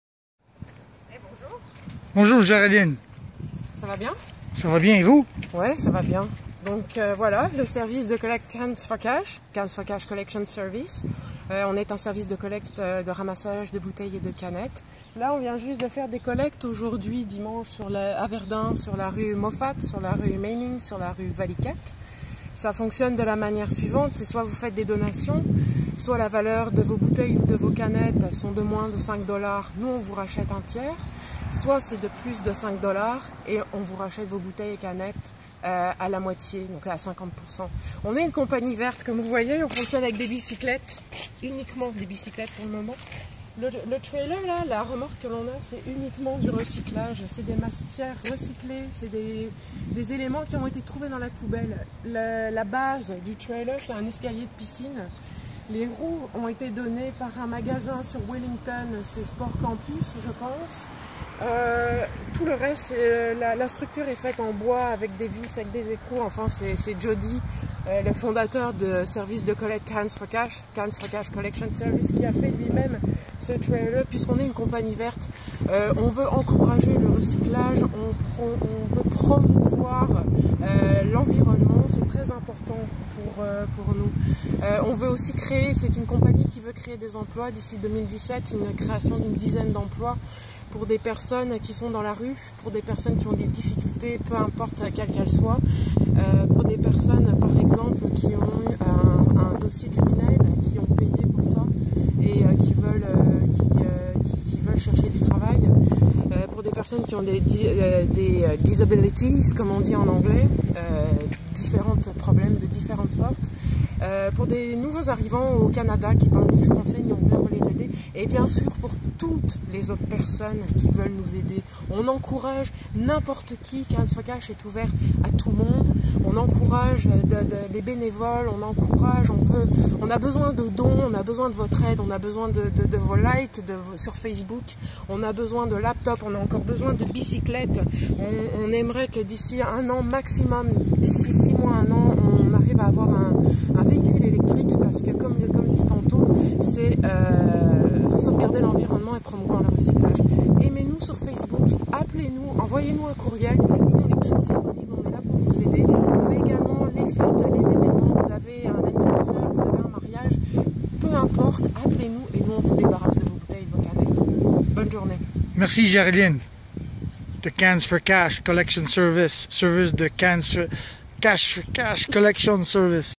Service de Collecte Cans4Cash explained in French by G
G tells us about Service de Collecte Cans4Cash. How it works and why it was created. Service de Collecte Cans4Cash was created by a person with disabilities for people with disabilities, the newly arrived to Canada, the less than fortunate, the homeless, those who made and paid for past mistakes in life, and everyone else who is a hard worker and who is in need of a job. (I love this video in particular because G, who is a perfect creature, makes a grammatical error in French) and I caught it on video.